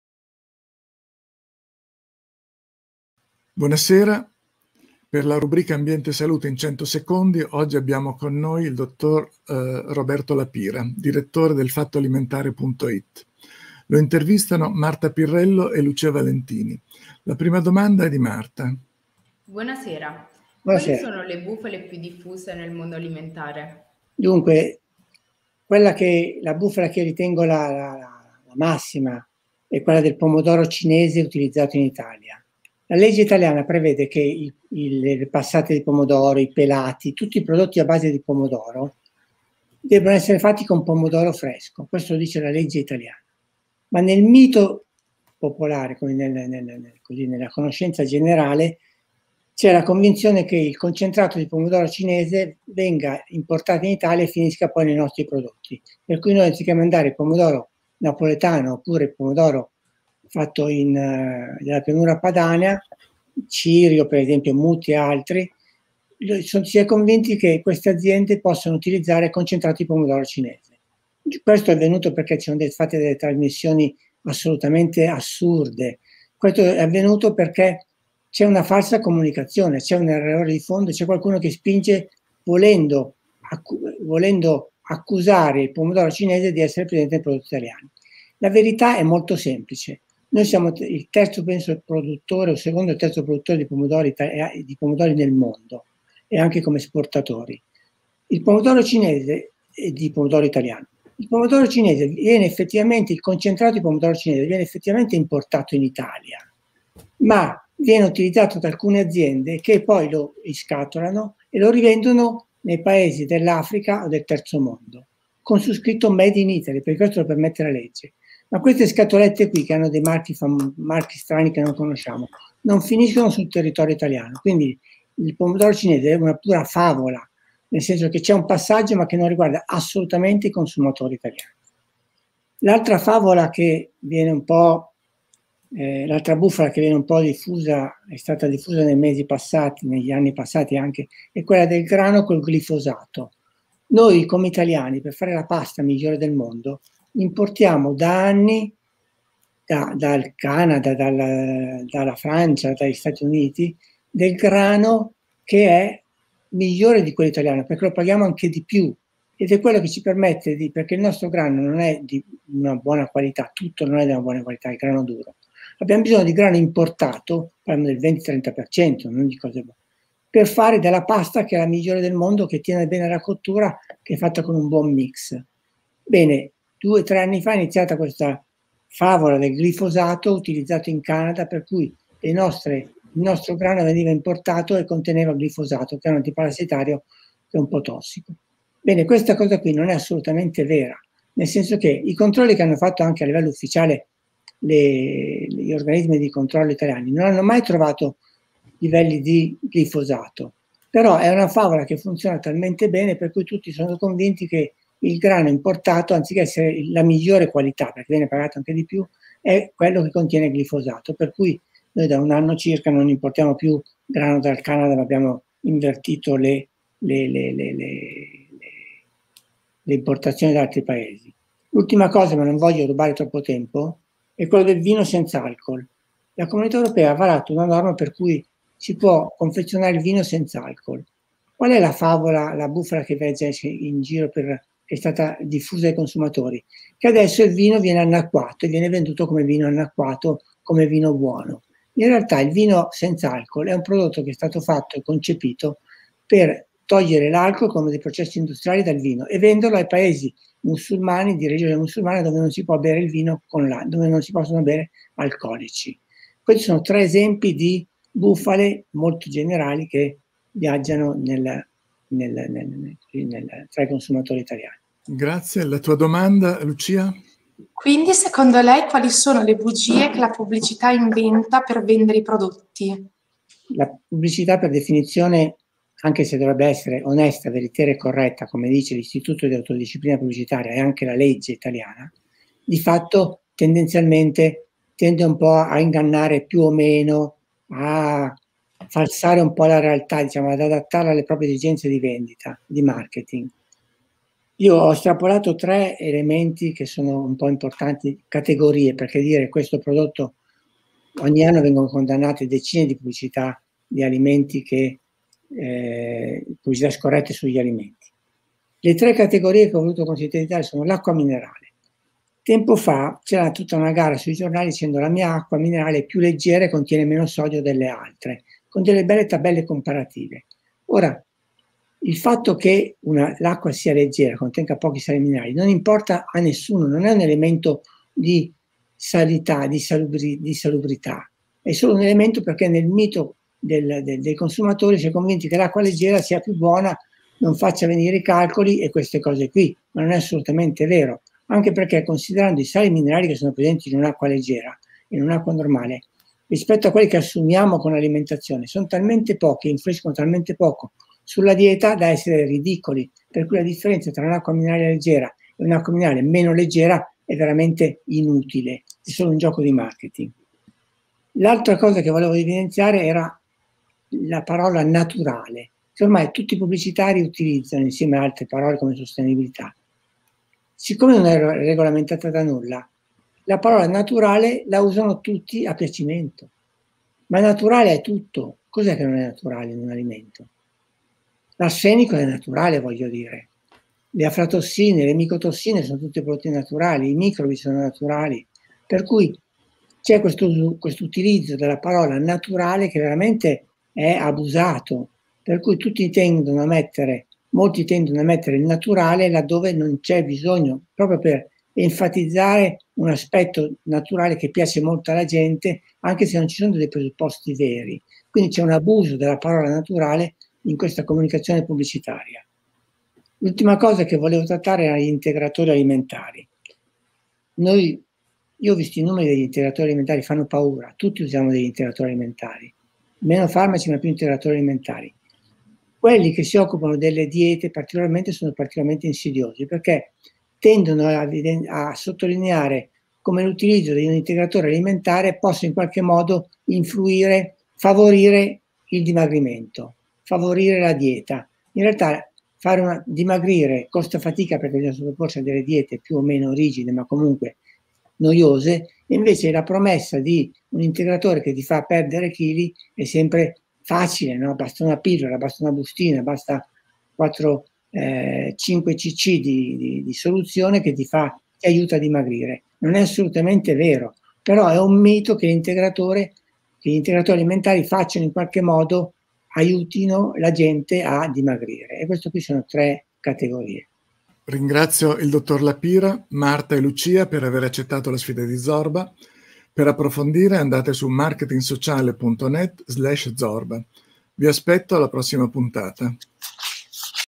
Roberto La Pira a "Ambiente e Salute in 100 secondi". A cura di Giuseppe Fattori
Le bufale nel mondo alimentare.
Roberto La Pira, Direttore de ilfattoalimentare.it, intervistato da Marta Pirrello e Lucia Valentini – Marketing Sociale APS.
Festival della salute - Zorba